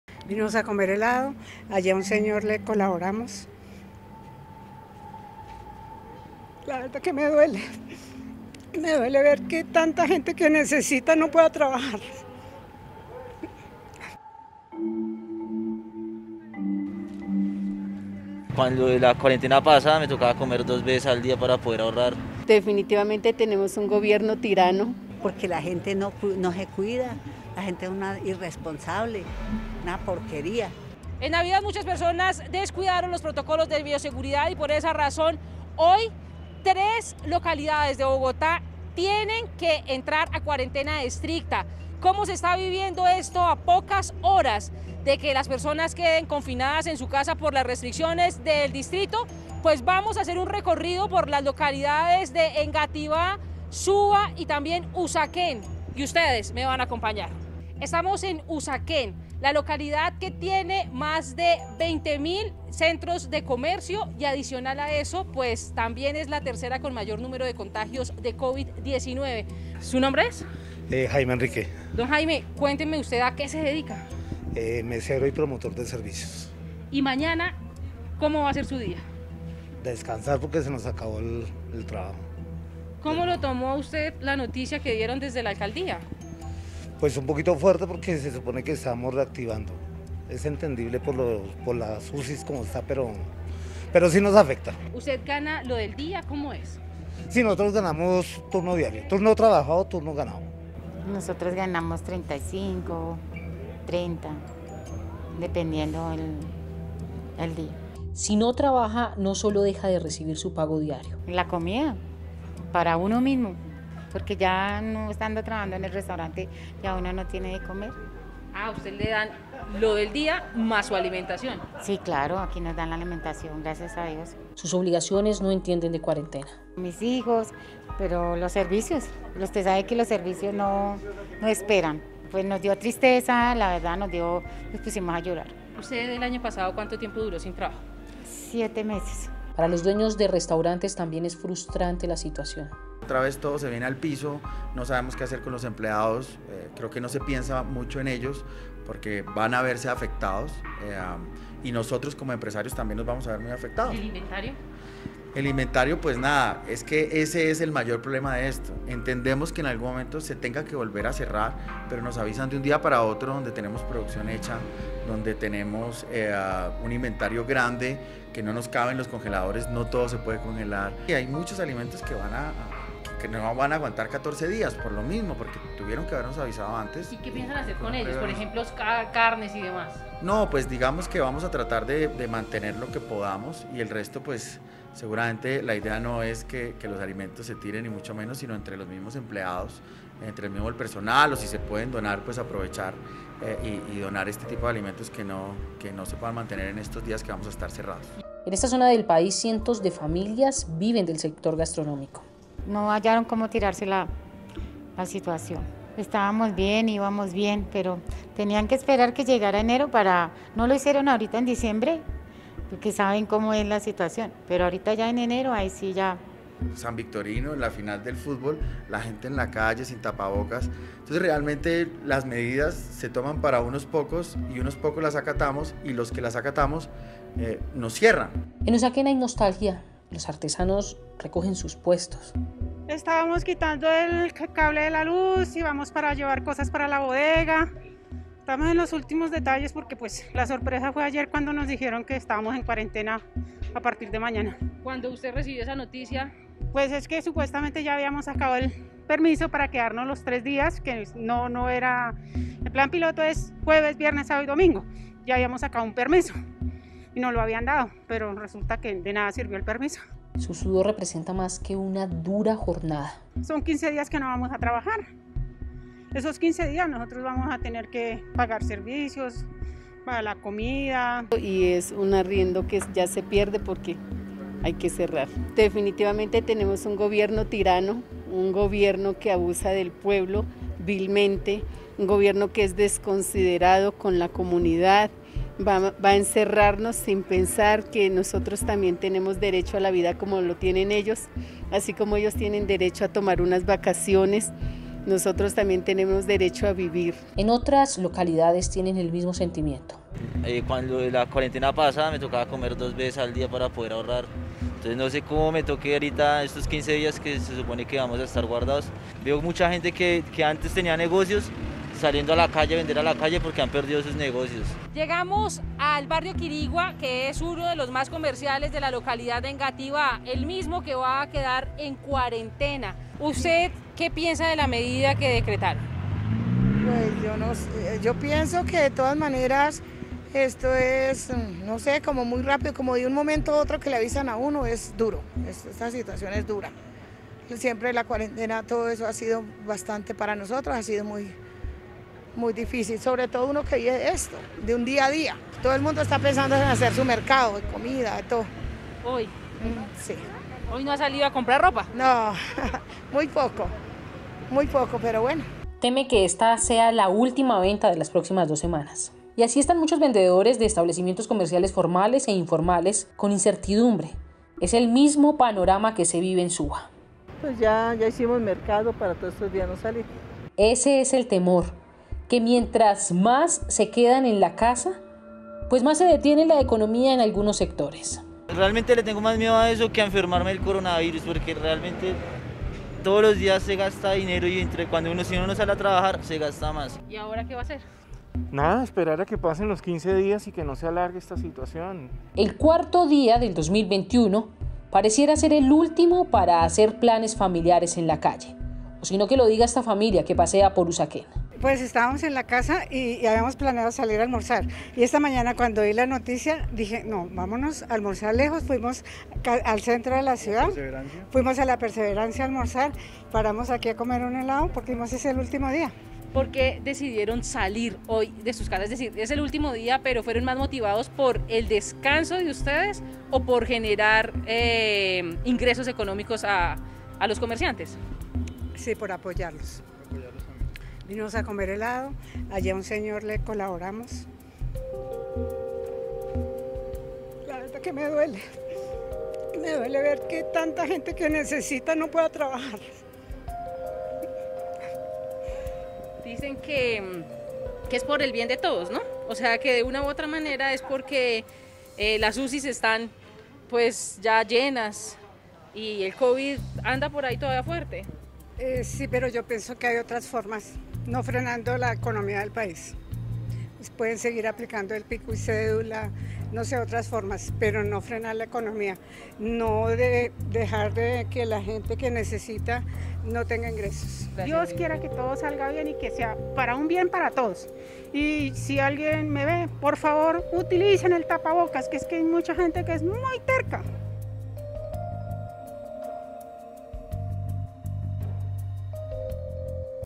Mujer rompió en llanto luego de que se decretara cuarentena en Bogotá | Semana Tv
El ver a comerciantes cerrando sus negocios horas antes de iniciar la cuarentena estricta afectó a más de un bogotano, no solo a propietarios y trabajadores. Una mujer en Usaquén no pudo contener el llanto luego de que la Alcaldía decretara la nueva medida.